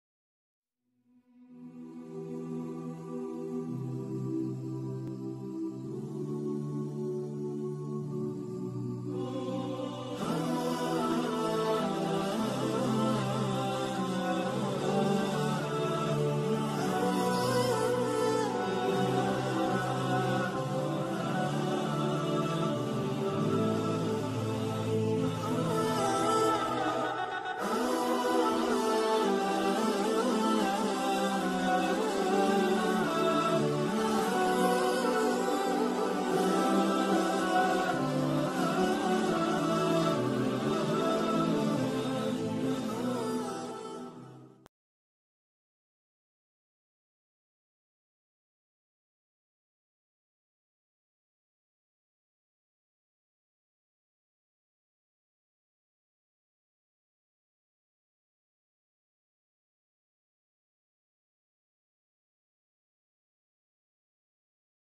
HAJJ 2020 | Sanitization of Kaabah Masjid Al Haram | Sterilization of Al Haram Makkah for Hajj 2020
Before Hajj the Sterilization process is start at Al Haram Makkah due to strict safety measures against COVID19. May Allah subahanahutallah save every one from this pandemic.